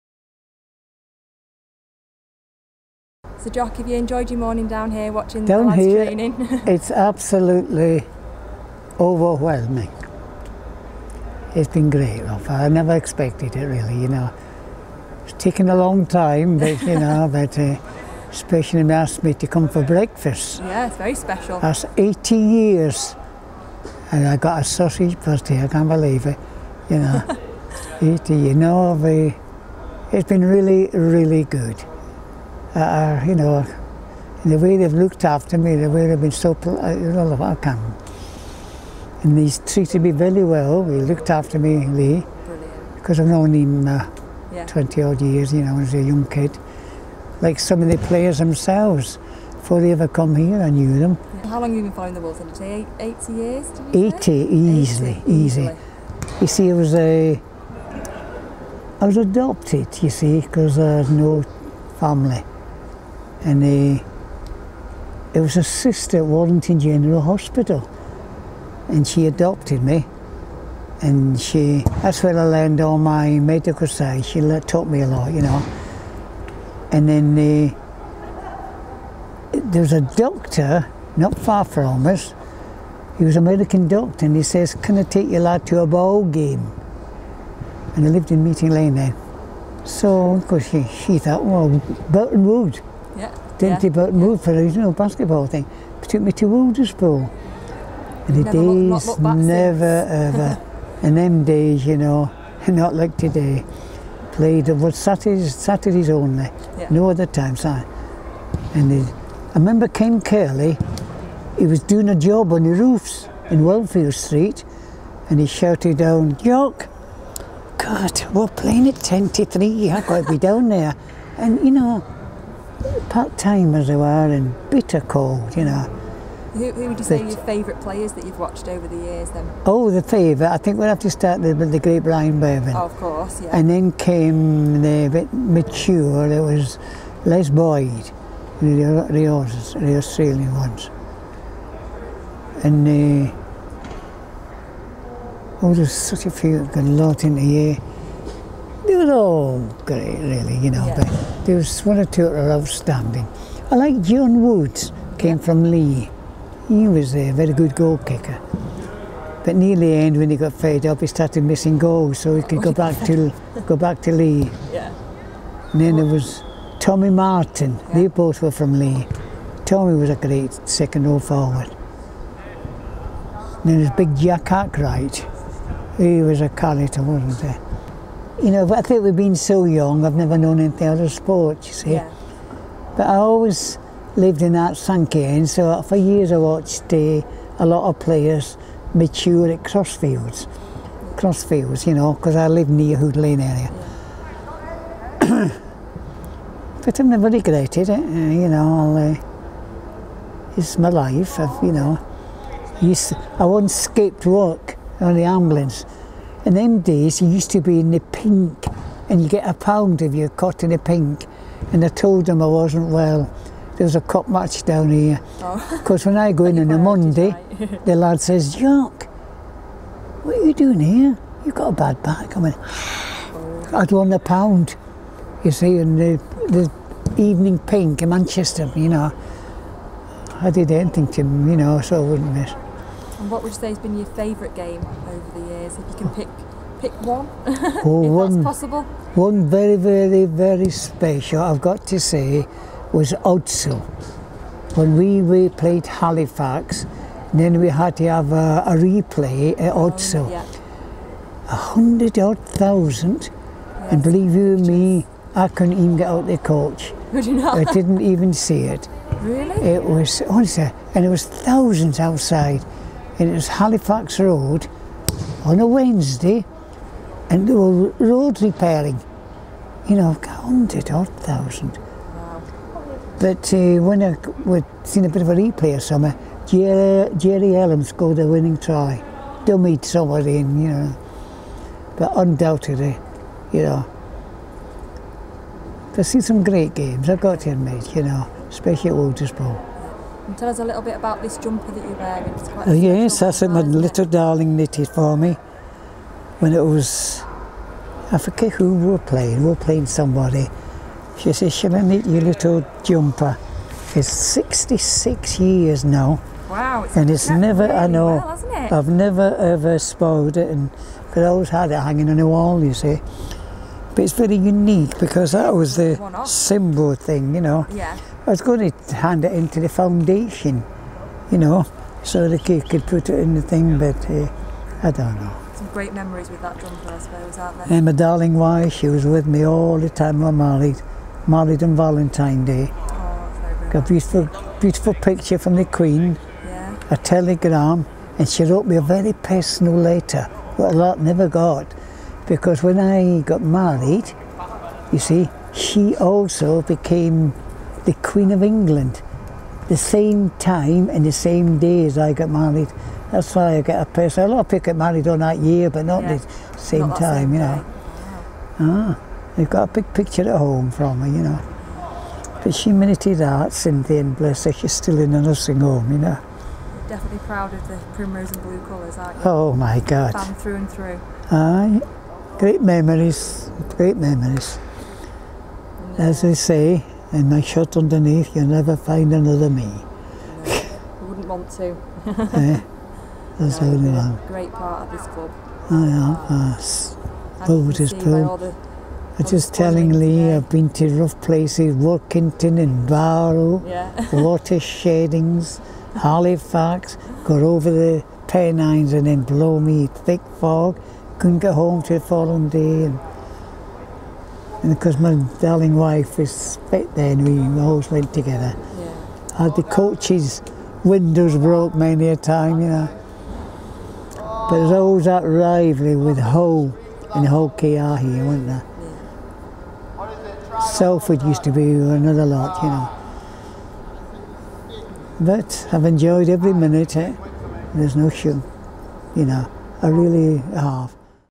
Jock is a Wire....
.and always will be.

Lifelong Wire fan James Patrick Maguire - or 'Jock' to everyone associated with the Warrington club - enjoyed a breakfast morning with his friends at the Wolves this week.

Despite having stomach cancer and having been given only a few weeks to live Jock is still the life and soul of the party. Here we speak to the man himself about his passion for the Wolves taking a trip down memory lane.